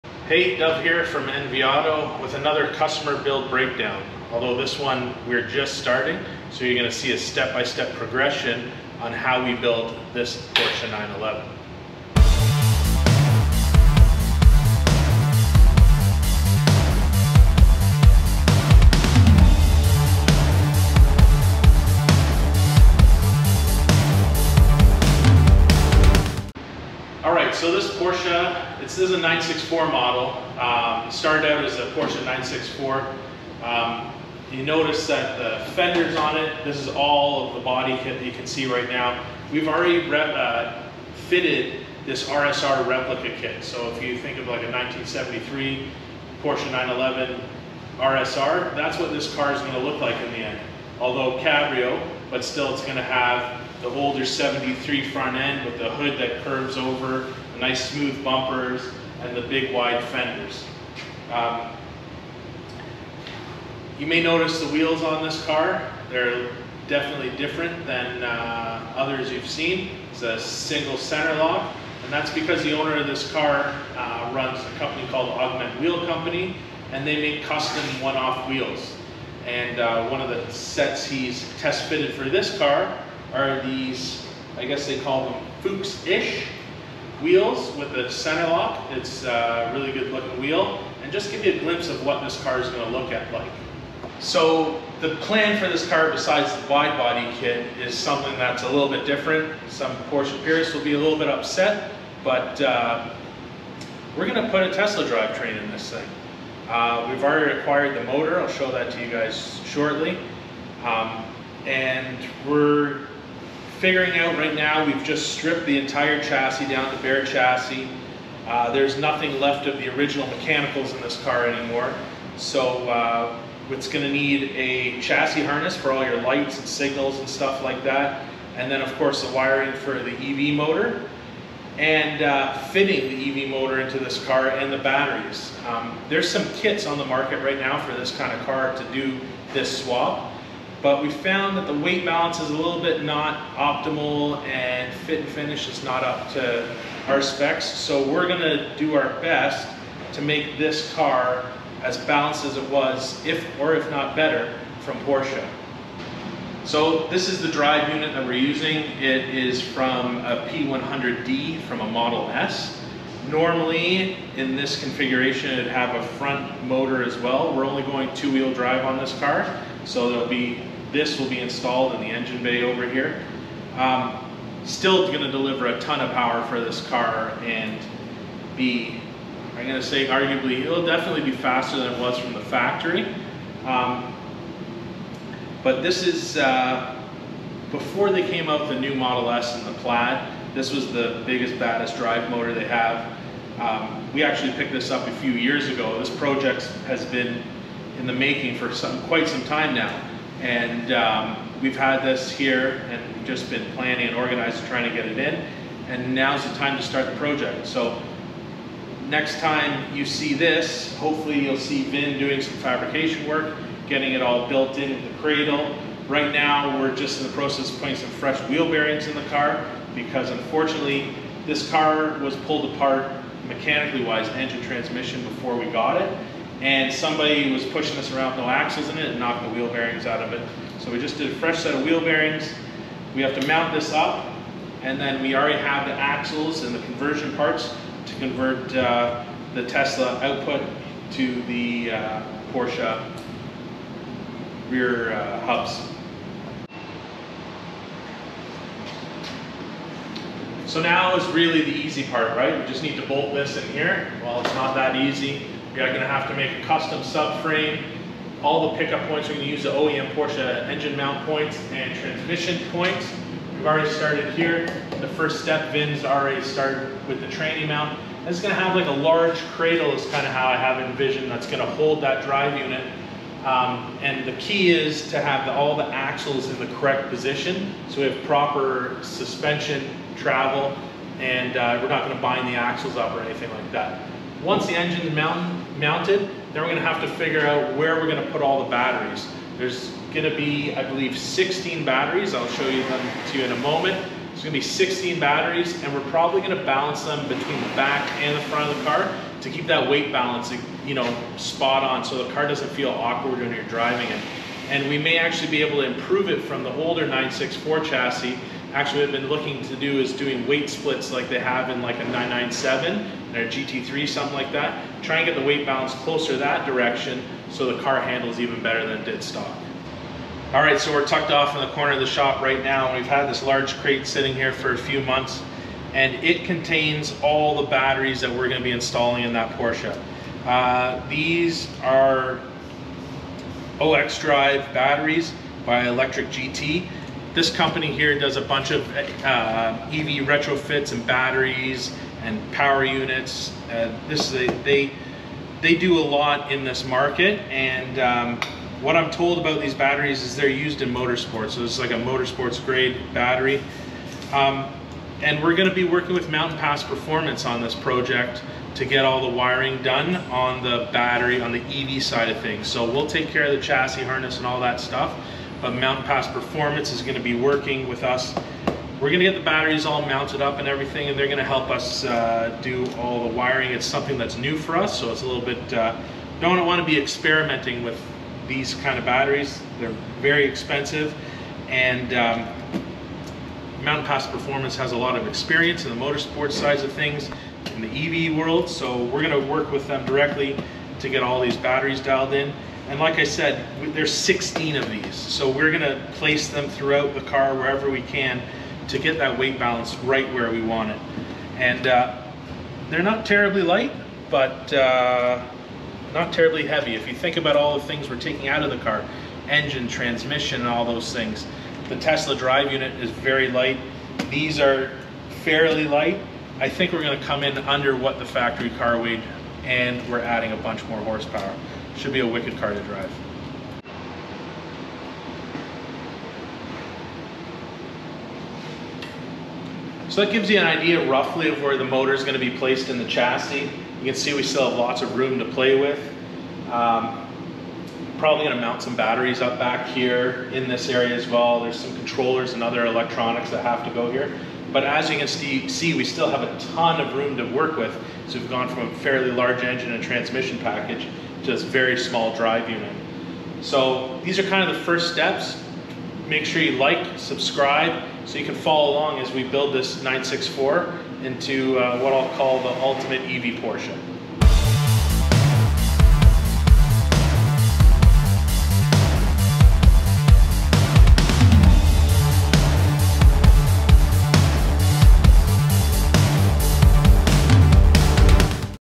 Tesla swapping a Porsche 964
On to the next build project here at the shop and this one is sure to upset many. We are installing a Model S P100D drive unit in a Porsche 964. Not only is this getting a EV drive unit but we have also cut up the car to add a RSR body kit. That is right we are back dating the looks and up dating the powerplant. This will swap will take some time and we are enlisting the help of our friends @MountainPassPerformance